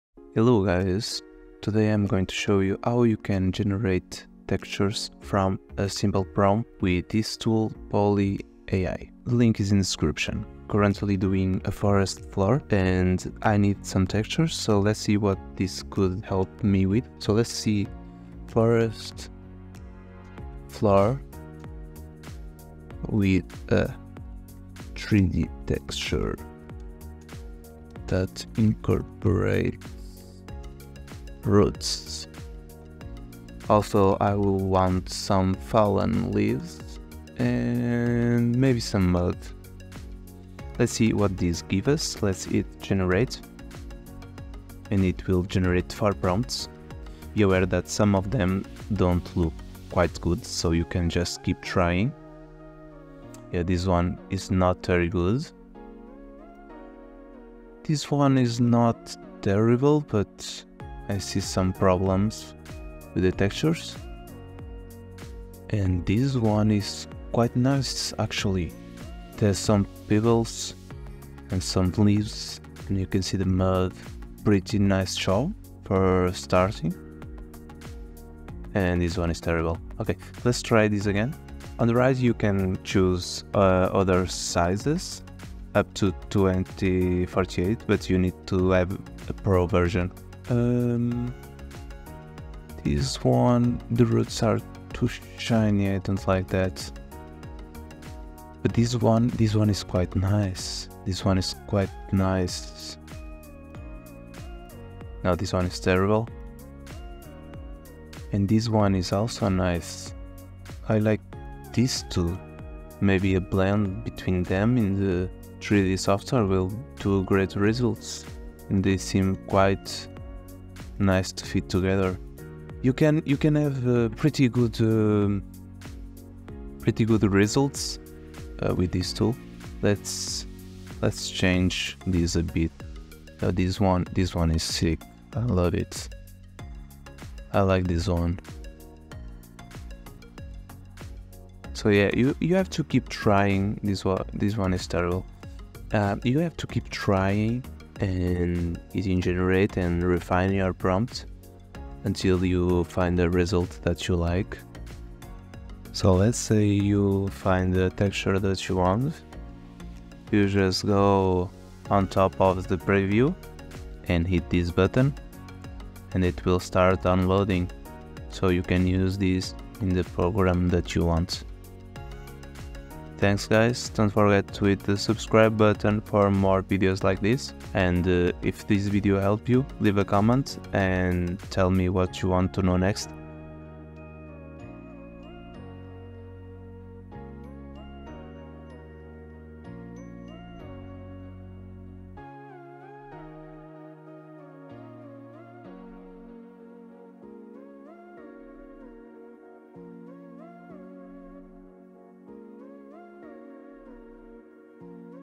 How to Create Textures for games with A.I. for free
Unleash the Power of AI in Game Development and Create Stunning, Free textures
In this groundbreaking video, we explore the cutting-edge realm of AI-generated textures for your 3d models, a game-changer for indie developers and professional studios alike. Discover how to harness AI tools without any cost. This tutorial guides you through the process of generating textures  all with the precision and intricacy that only AI can offer. Ideal for enhancing the visual fidelity of your game environments. Dive in and transform your game development workflow with the power of AI!